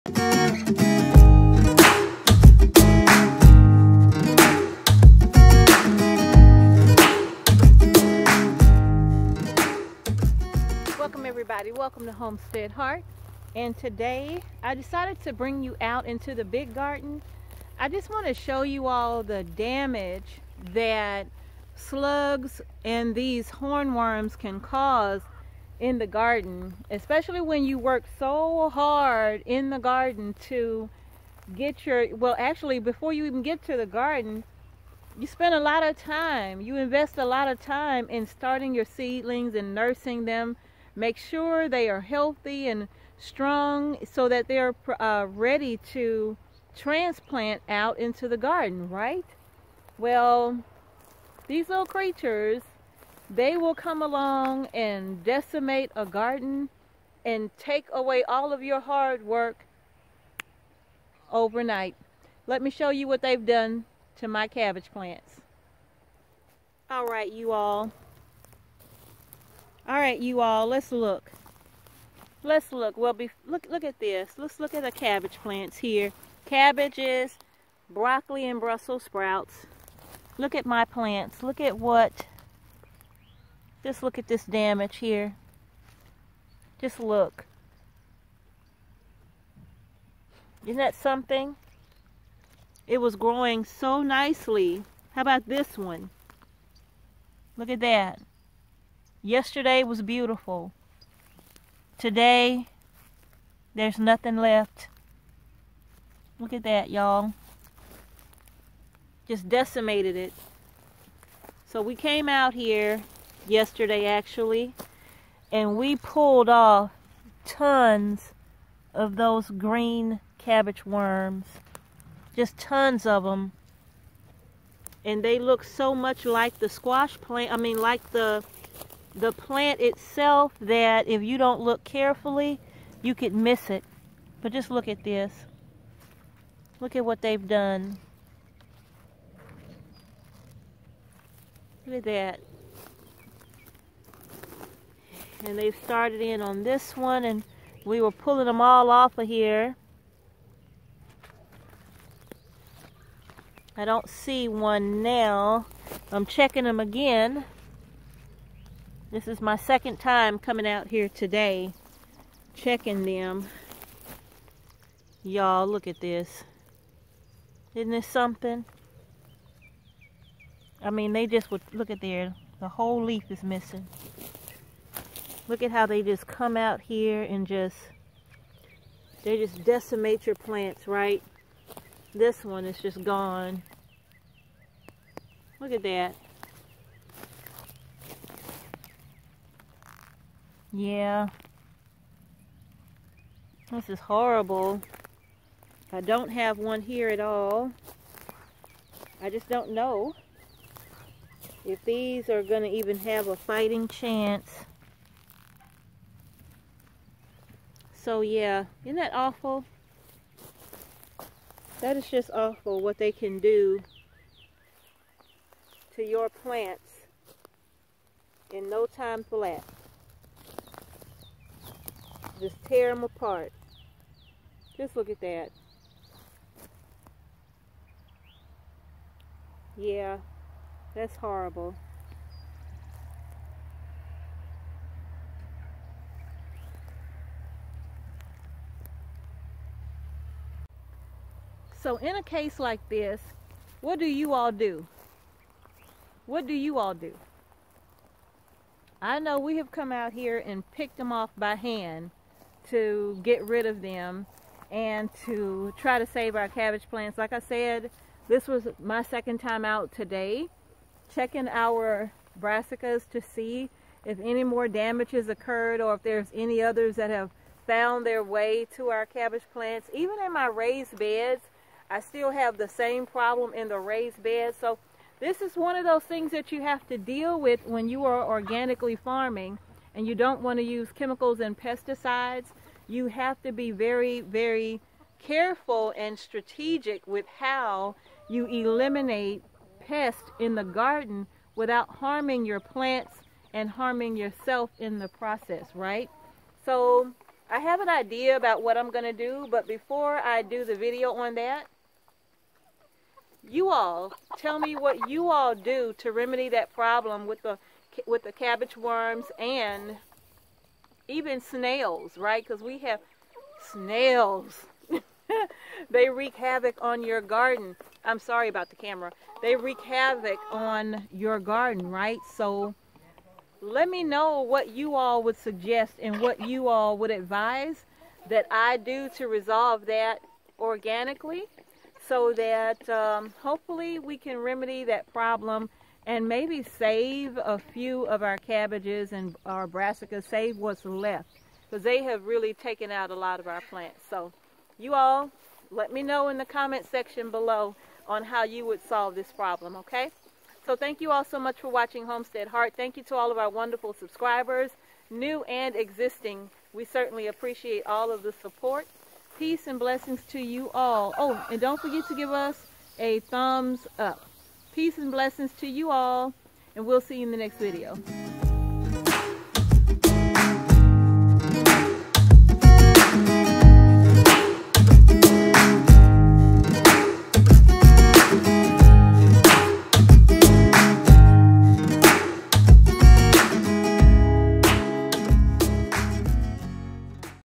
THE DAMAGE CABBAGE WORMS CAN CAUSE
Cabbage worms and snails have done major damage to our garden. Because we have an Organic Farm, we are not using chemical pesticides. However, we will find a way to combat these annoying little creatures.